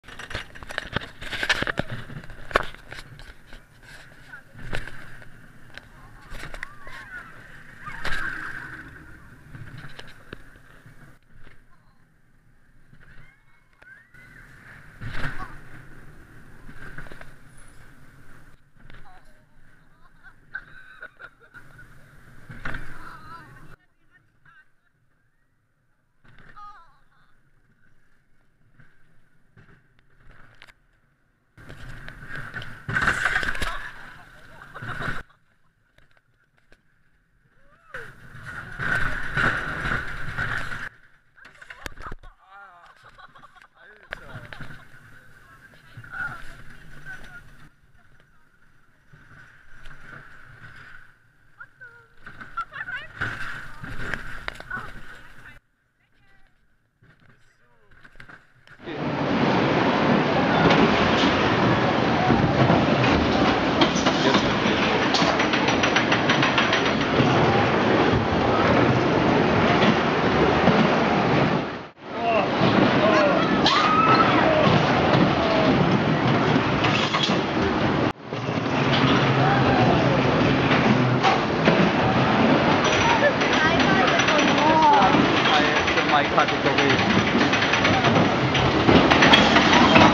Holly´s Wilde Autofahrt | Holiday Park | Onride 2014 [HD]
Beschreibung vom Coaster:
Holly´s Wilde Autofahrt ist eine herkömmliche Wilde Maus aus dem Holiday Park in Hassloch. Sie stellt eine Autofahrt dar, wobei man jedoch einige Verkehrsschilder missachtet.

Statistiken:
-Hersteller : Maurer AG
-Höhe : 15 Meter
-Länge : 370 Meter
-Inversionen : 0
-Max. Geschw. : 45 km/h

TheCoasterRides

Description:
This coaster is just a normal wild mouse coaster with a "Street theming". It´s standing in Holiday Park Hassloch in Germany.

Stats :
-Manufacturer : Maurer AG
-Heigth : 15 meter [50 feet]
-Length : 370 meter [1213 feet]
-Inversions : 0
-Max. Speed : 45 km/h [28 mph]

TheCoasterRides
Got any questions or inspirations? Just write us a message.